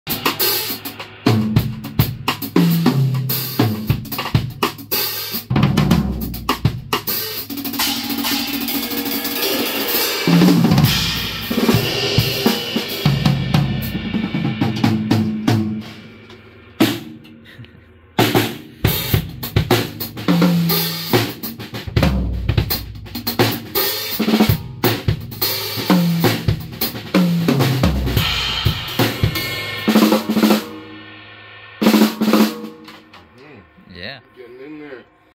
Larnell Lewis is here !
Just off the plane and trying the drums for tomorrow's session. New album on the way !